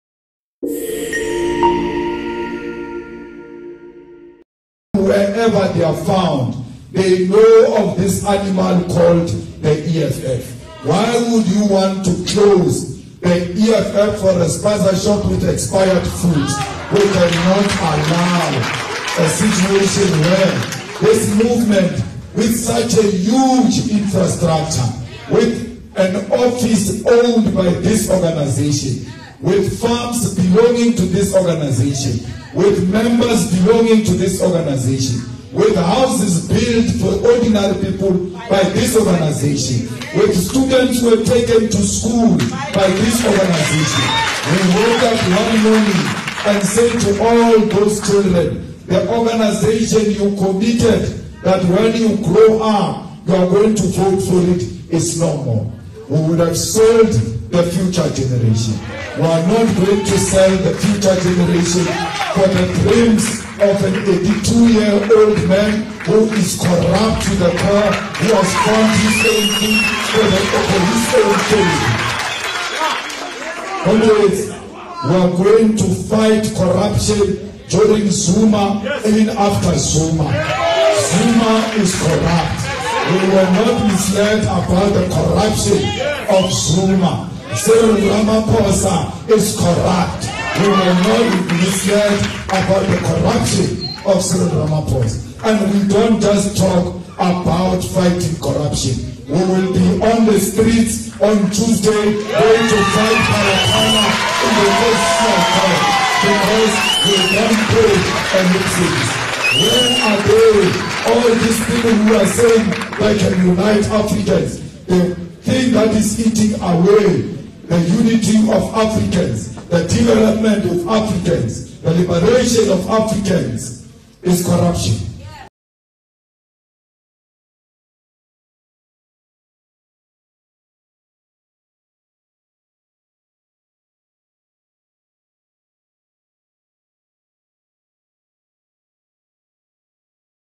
Julius Malema: 'Zuma and Ramaphosa are Corrupt, We'll Fight Them in the Streets
"We are not going to sell the future generation for the dreams of an 82-year-old man who is corrupt to the core. We are going to fight corruption under Jacob Zuma and even after Zuma." - EFF President Julius Malema

"Zuma is corrupt; we were not misled about the corruption of Zuma. Cyril Ramaphosa is corrupt; we were not misled about the corruption of Cyril Ramaphosa. And we don't just talk about corruption - we will be on the streets on Tuesday to fight Phala Phala." @Julius_S_Malema